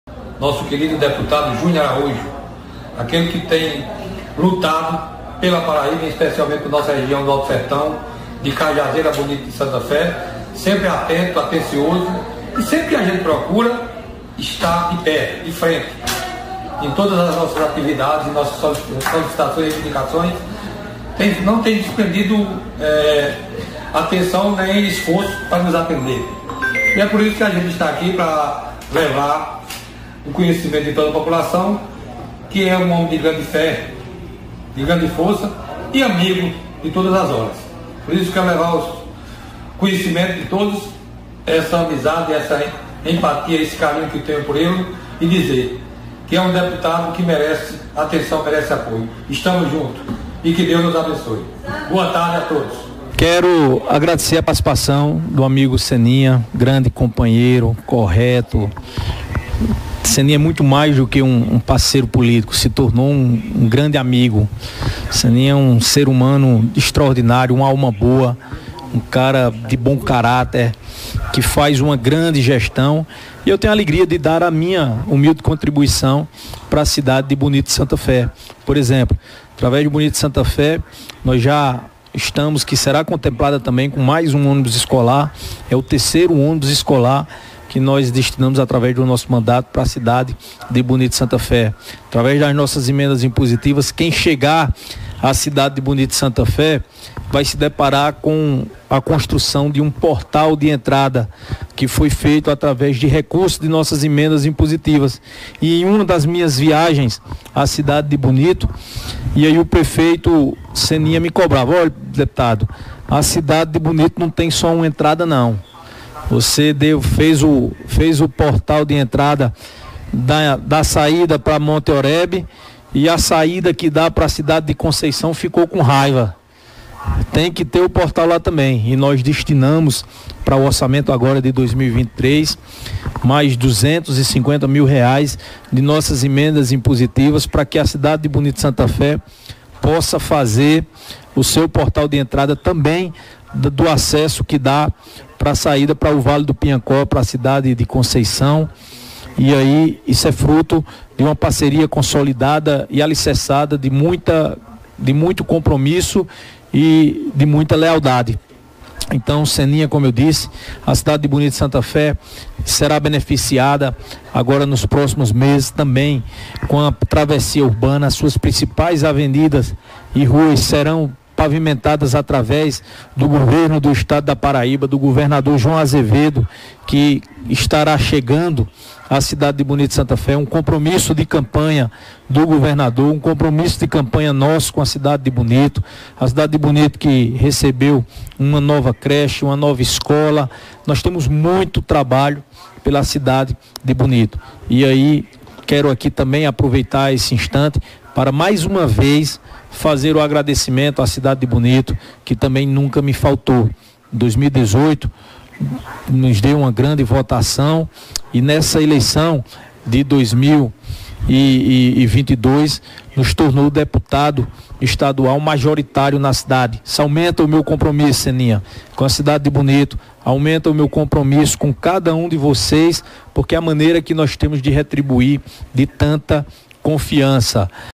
Júnior Araújo destaca investimento de R$ 250 mil para construção de portal em Bonito de Santa Fé
Parceiro de Ceninha Lucena, Júnior Araújo destaca investimento de R$ 250 mil para construção de portal de entrada no município
17/02/2023